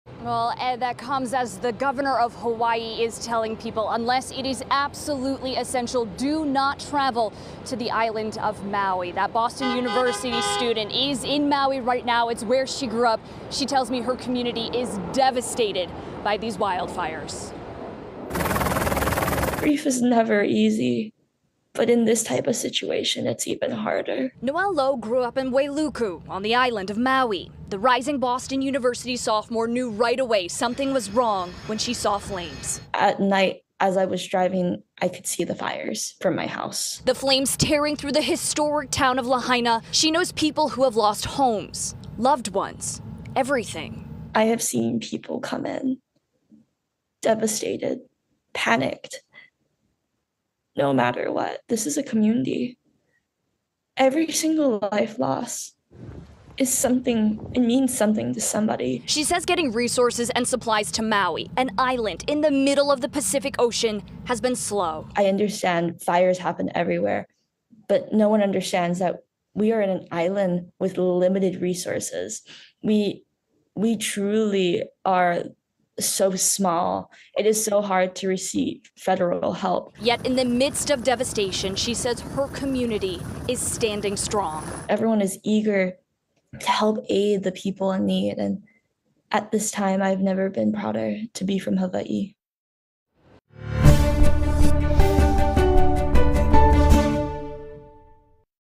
BU student describes fires near Maui home
Noelle Lo, a Boston University student, was born and raised in Maui and is there now. She said she could see the flames burning from her house.